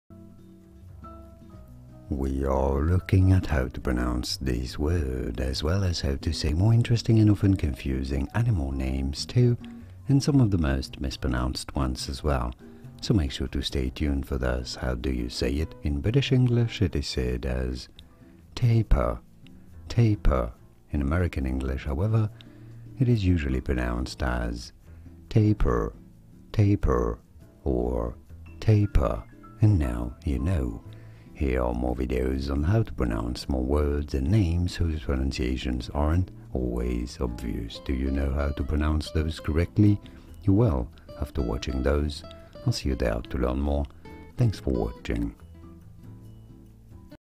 How to Pronounce Tapir? (CORRECTLY)
This video shows you how to pronounce Tapir (pronunciation guide).

Information & Source
A tapir is a large, herbivorous mammal, similar in shape to a pig, with a short, prehensile nose trunk. Tapirs inhabit jungle and forest regions of South and Central America, with one species inhabiting Southeast Asia. Wikipedia Source
Family: Tapiridae; Brünnich, 1772
Class: Mammalia
Phylum: Chordata
Order: Perissodactyla
Kingdom: Animalia
Gestation period: Malayan tapir: 13 months, South American tapir: 13 months, Mountain tapir: 13 months